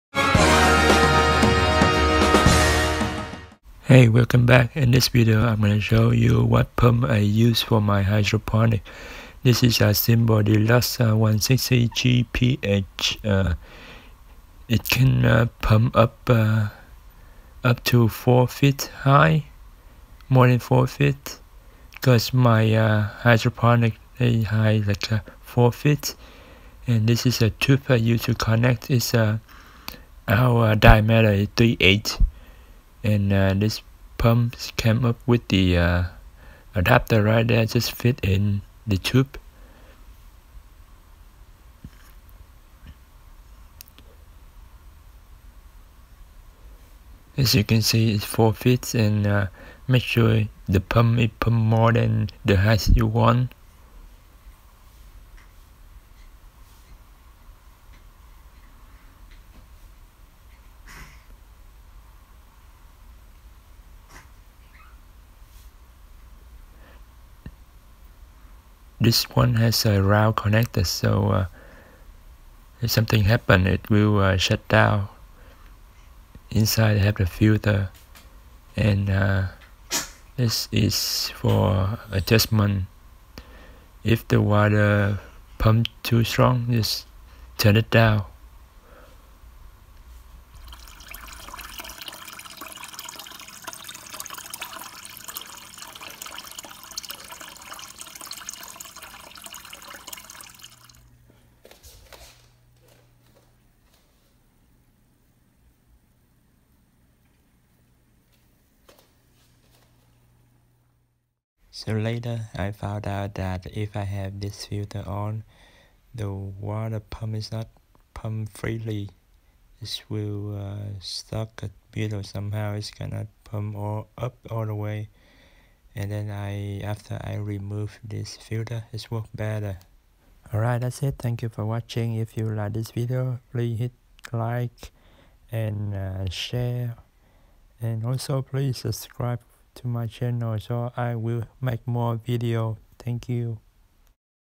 I used this pump for my Hydroponic - Simple Deluxe 160gph
Water submerse pump for hydroponics. Please consider the GPH power before choosing you pump.
Here are some ON SALE good pumps